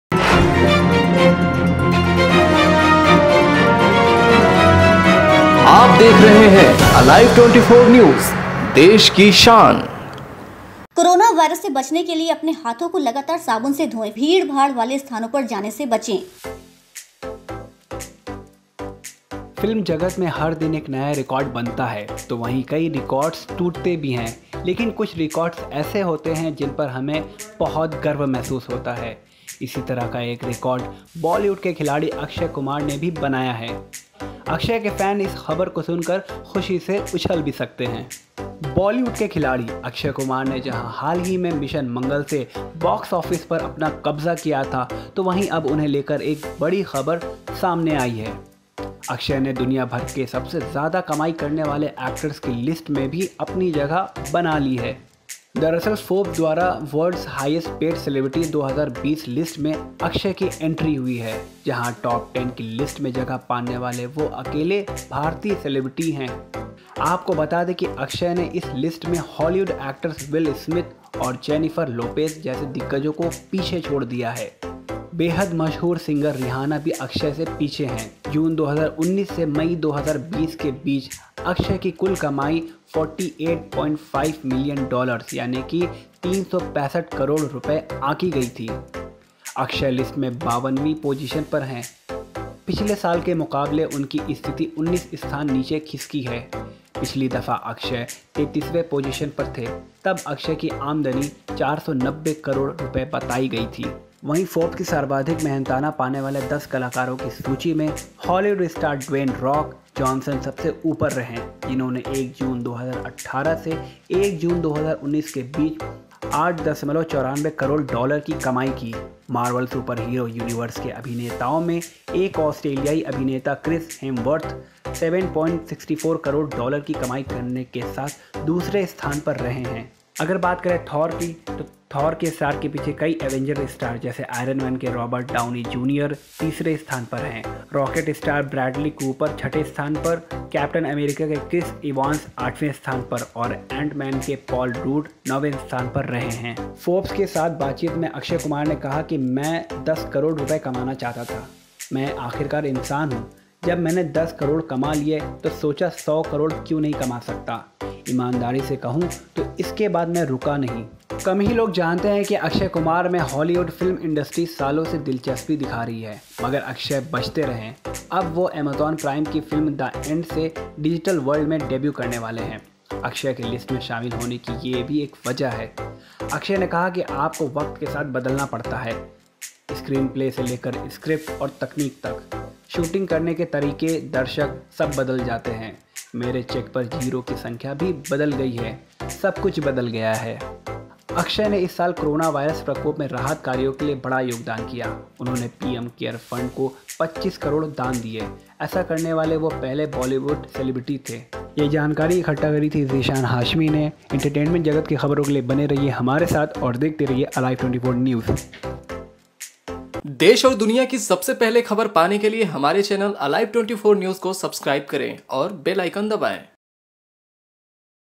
Akshay Kumar फिर बने देश की सबसे कमाऊ हस्ती, जानिए कितनी है Income
फिल्म जगत में हर दिन एक नया रिकॉर्ड बनता है तो वहीँ कई रिकॉर्डस टूटते भी है, लेकिन कुछ रिकॉर्डस ऐसे होते है जिन पर हमें बहुत गर्व महसूस होता है इसी तरह का एक रिकॉर्ड बॉलीवुड के खिलाड़ी अक्षय कुमार ने बनाया है और अक्षय के फैंस इस खबर को सुनकर खुशी से उछल भी सकते हैं.
बॉलीवुड के खिलाड़ी अक्षय कुमार ने जहां हाल ही में 'मिशन मंगल' से बॉक्स ऑफिस पर कब्जा किया था तो वहीं अब उन्हें लेकर एक बड़ी खबर सामने आई है. अक्षय ने दुनिया भर के सबसे ज्यादा कमाई करने वाले एक्टर्स की लिस्ट में भी अपनी जगह बना ली है.
दरअसल फोर्ब्स द्वारा जारी World’s Highest-Paid Celebrities 2020 लिस्ट में अक्षय की एंट्री हुई है। जहाँ टॉप 10 की लिस्ट में जगह पाने वाले वो अकेले भारतीय सेलेब्रिटी हैं।